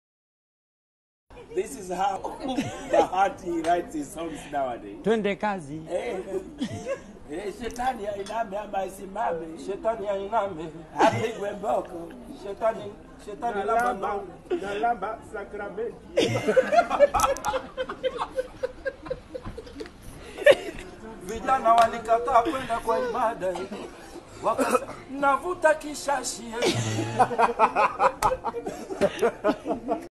KENYAN COMEDIANS MAKING FUN OF SINGER BAHATI NEW STYLE OF MUSIC (JALANG'O DJ SHITTI OBINNA)
Is this is how Bahati makes his song nowadays? 😂😂